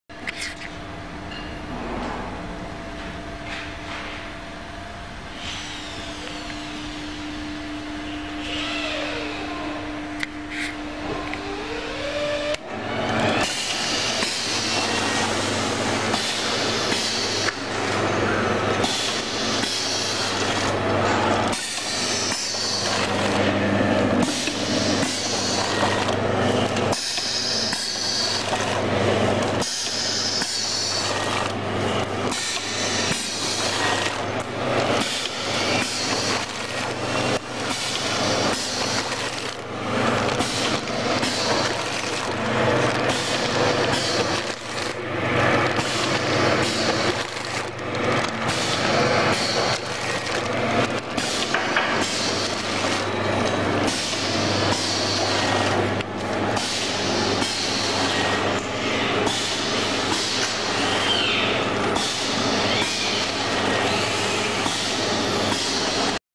Sauce & Paste Packing Machine
Suitable for packing bags, such as jam, honey, shampoo, chafing dish, bean paste, chili sauce, beef sauce, seasoning bags